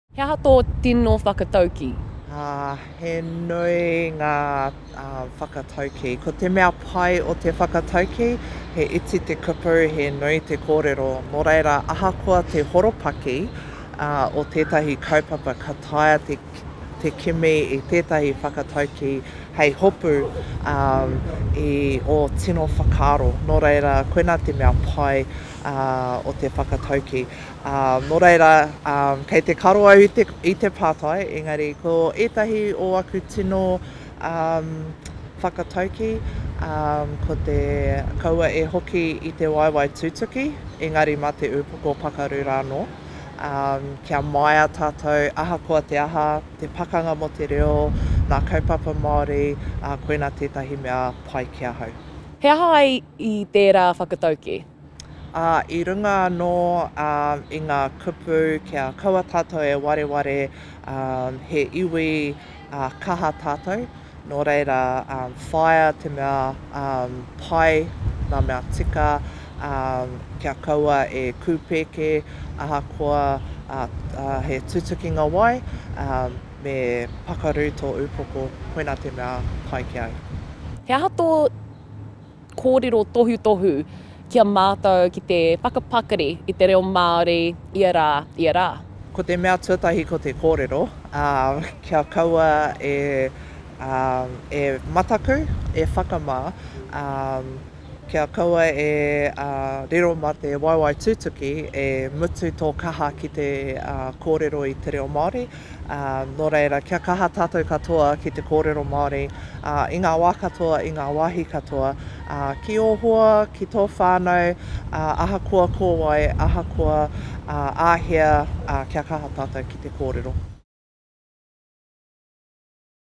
Te Wā Tuku Reo Māori | The Māori Language Moment with Rawinia Higgins and Meriana Johnsen | RNZ
RNZ's Meriana Johnsen speaks to Te Taura Whiri i te reo Māori chair Rawinia Higgins as part of the Māori Language Moment.